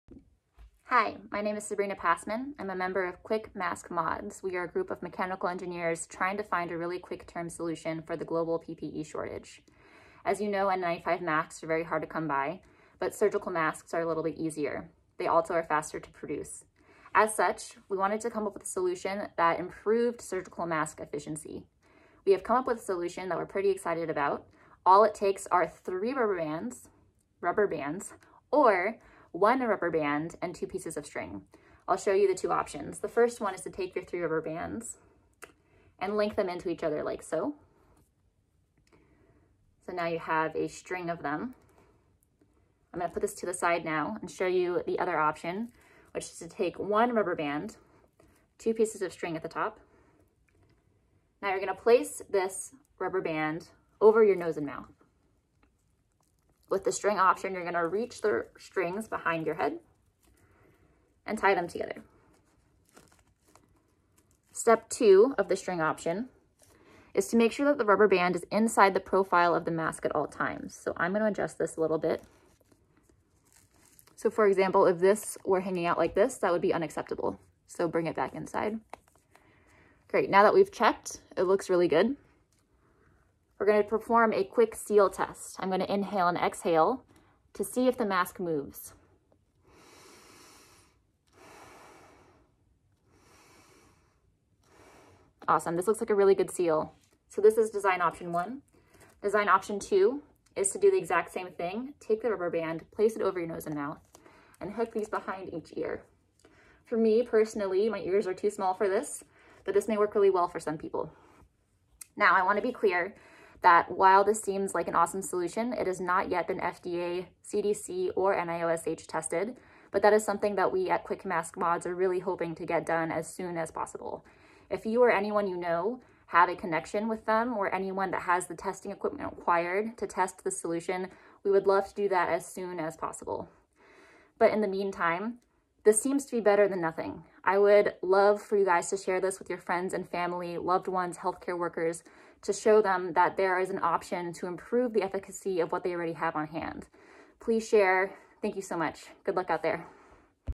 Quick Mask Mods
We are Quick Mask Mods, a group of mechanical engineers trying to solve the global PPE shortage. We have come up with a solution to improve efficacy of surgical masks. Please note, this has not been CDC/NIOSH or FDA approved, so please use at your own risk.